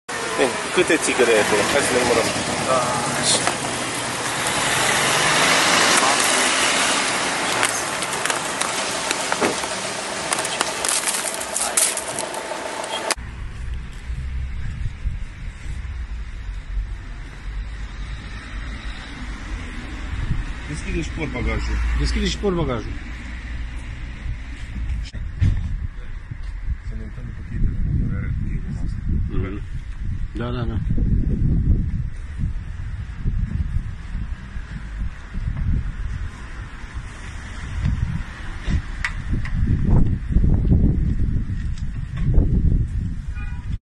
Tigari netimbrate
366 de pachete de țigări de contrabandă, confiscate de jandarmi